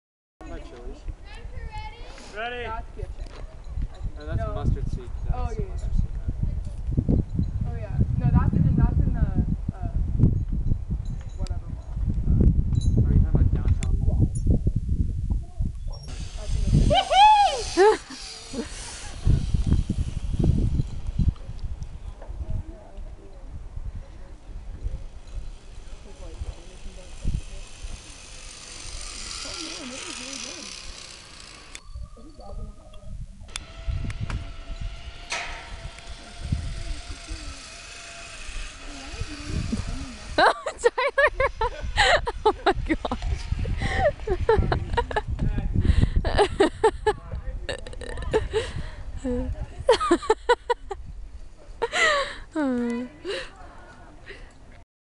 YOU GOT TO WATCH THIS...HILARIOUS. Adventure Dynamics Zipline
At 39 seconds you'll see where he thinks he is pretty funny lol